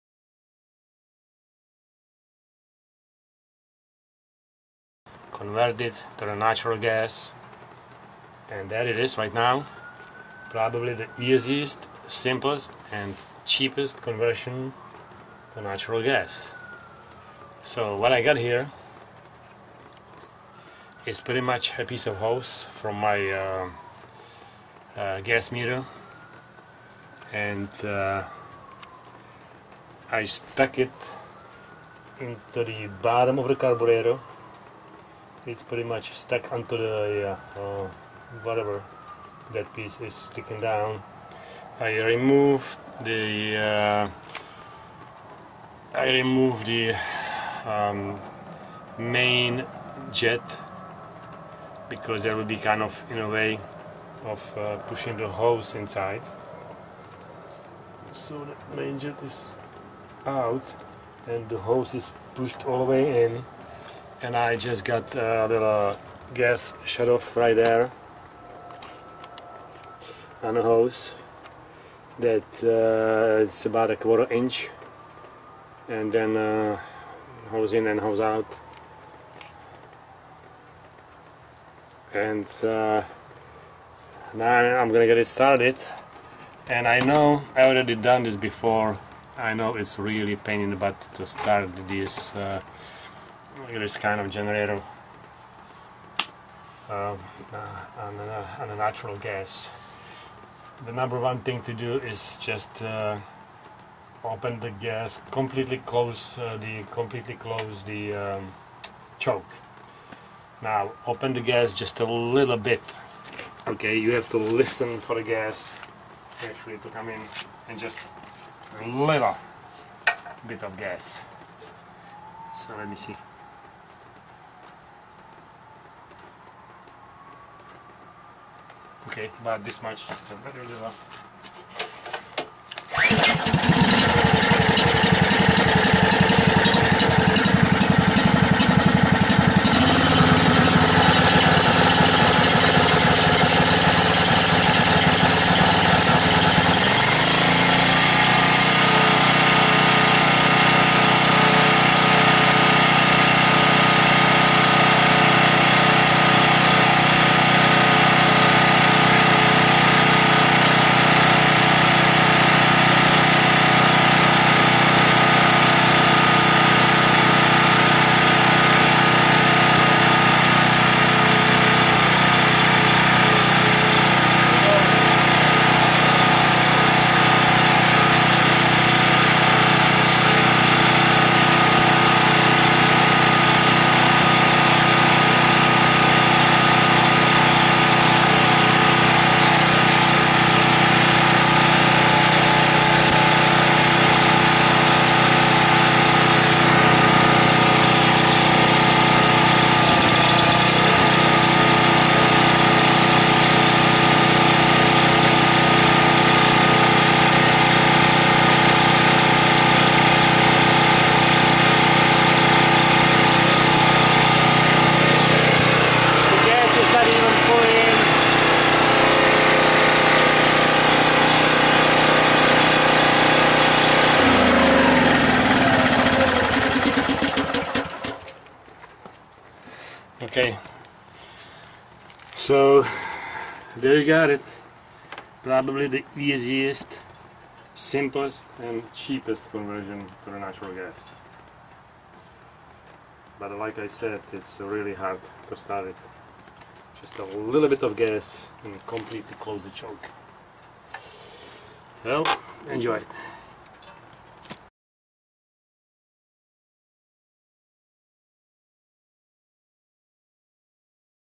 Easiest natural gas conversion
Easiest, simplest, cheapest natural gas conversion for gasoline generator.